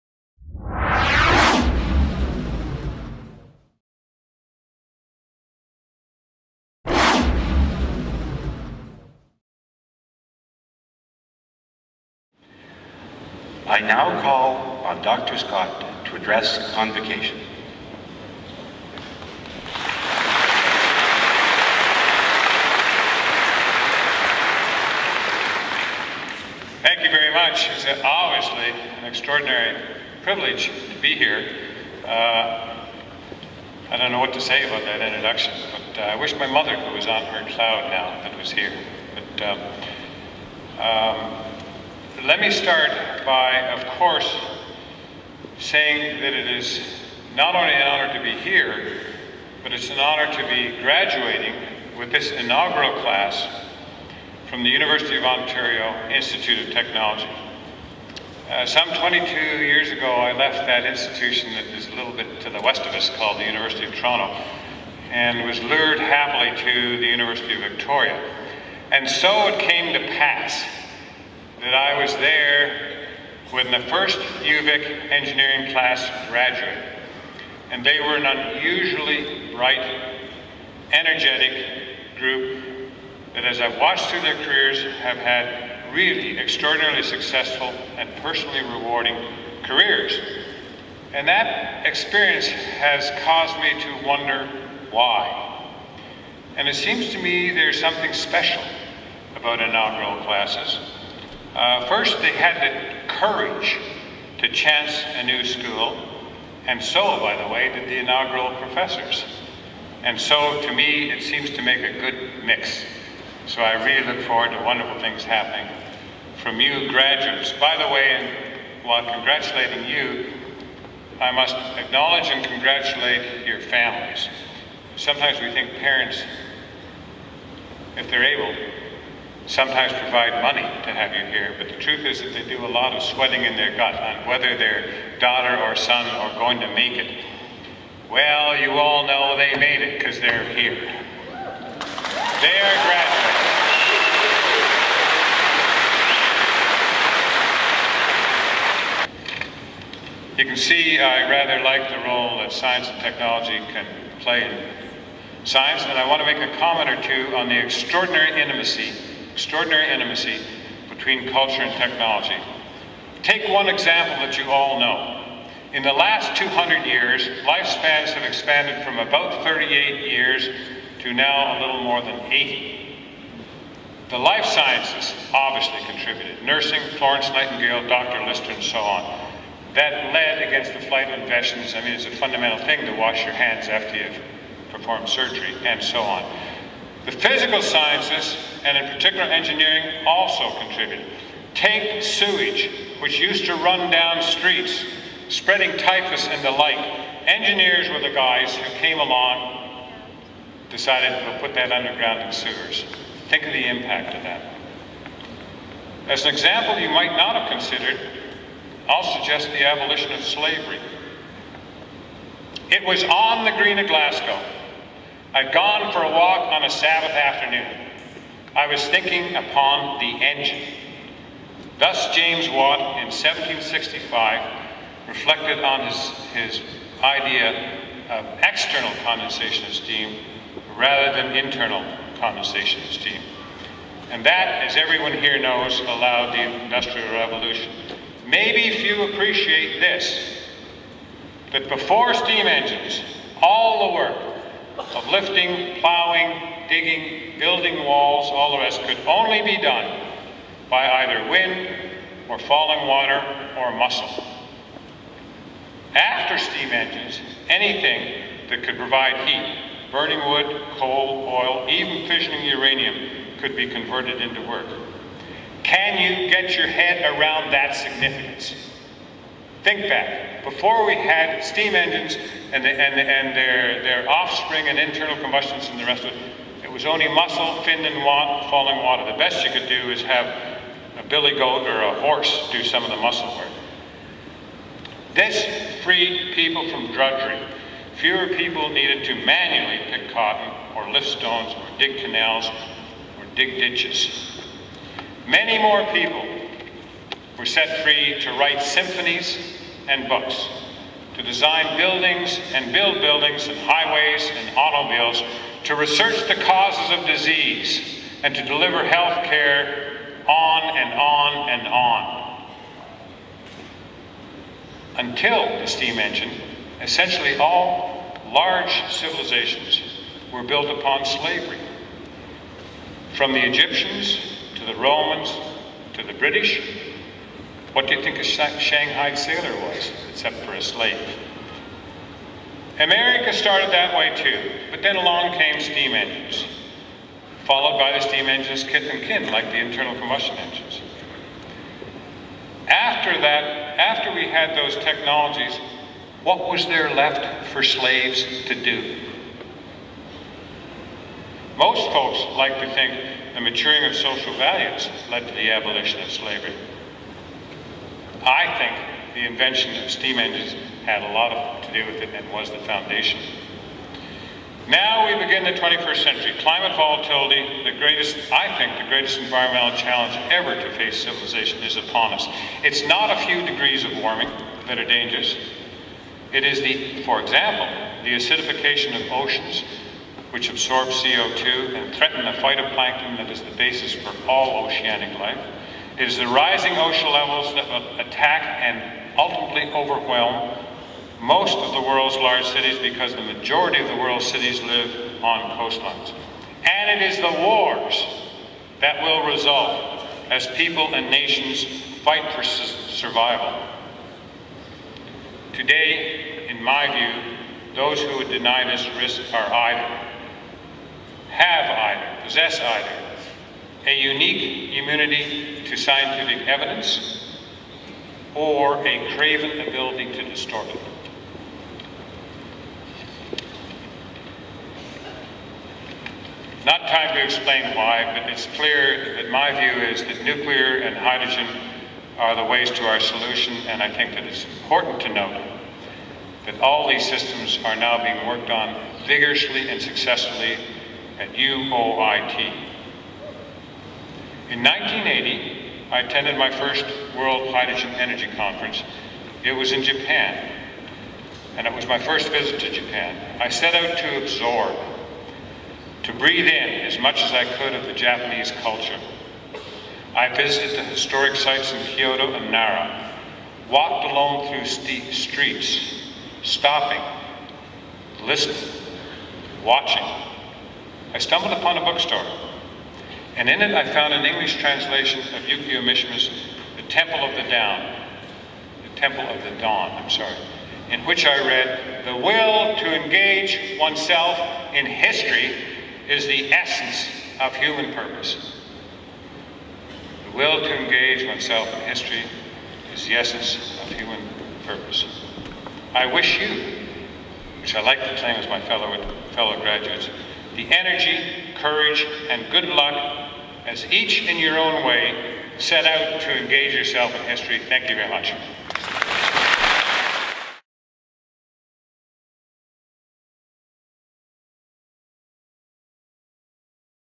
2007 UOIT Convocation - Dr. David Sanborn Scott
Conferred with an Honorary Doctor of Science, Dr. Scott is the founding director of the for more information.

Conferred with an Honorary Doctor of Science, Dr. Scott is the founding director of the Institute for Integrated Energy Systems at the University of Victoria. He has made numerous contributions to the study of hydrogen, is the author of the influential report, Hydrogen: National Mission for Canada and serves as a consultant to the federal government, major U.S. laboratories and multinational consulting firms. He is currently vice-president for the Americas of the International Association for Hydrogen Energy and was recognized by that body with the prestigious Jules Verne Award in 2006.

This YouTube account is maintained by the official Communications and Marketing Department of UOIT.